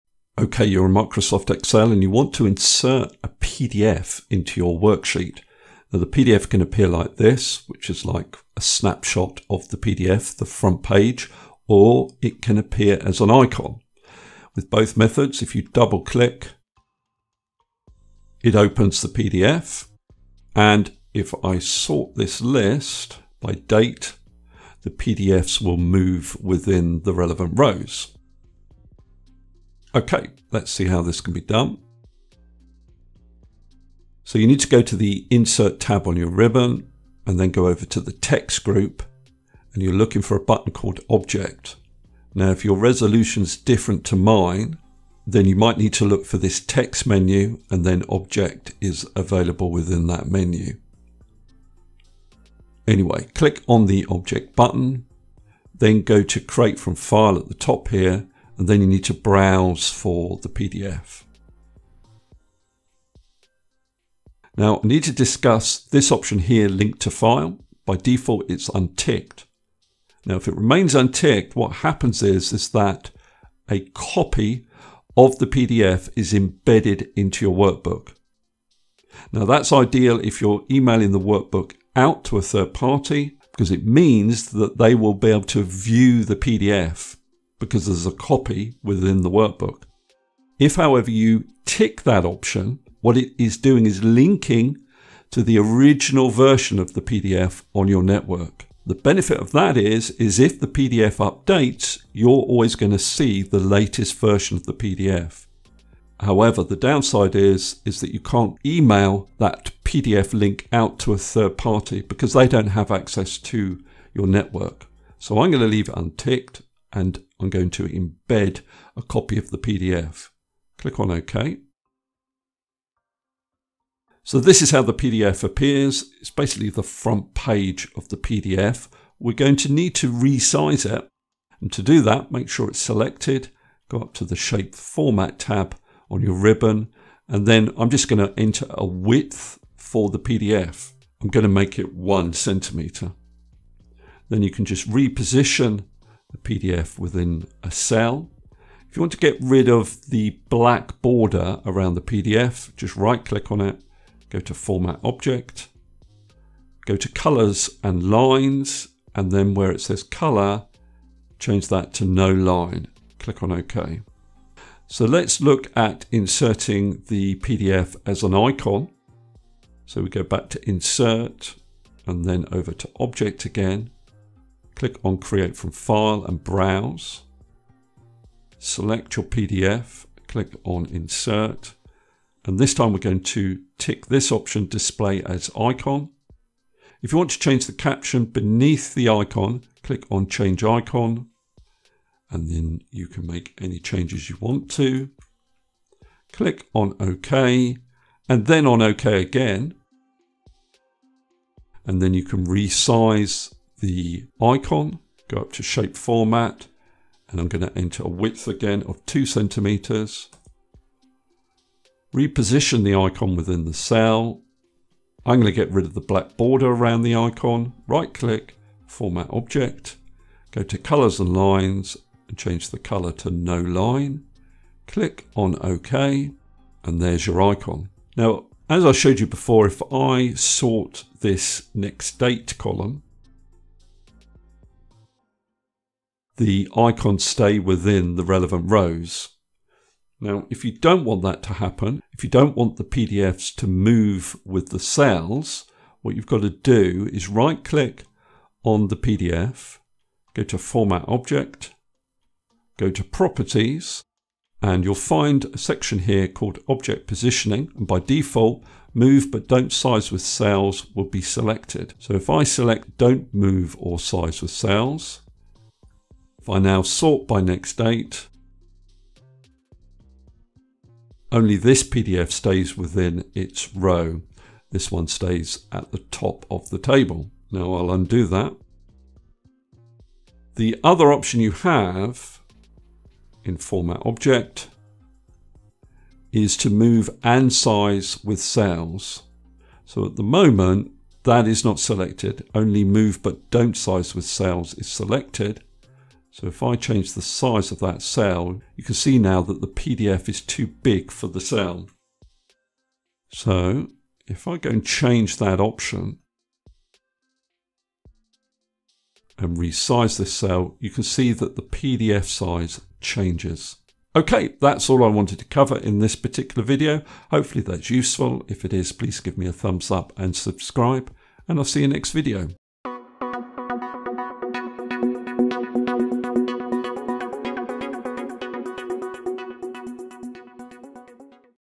How to Insert a PDF in Excel | Open or Link PDFs in an Excel Cell Without Software
In this Microsoft Excel video tutorial I explain how to embed or link a PDF in an Excel worksheet.

🕰️ Timestamps
00:00 Introduction
00:29 Insert PDF as front page
02:44 Insert PDF as icon
03:47 Move and size within cell options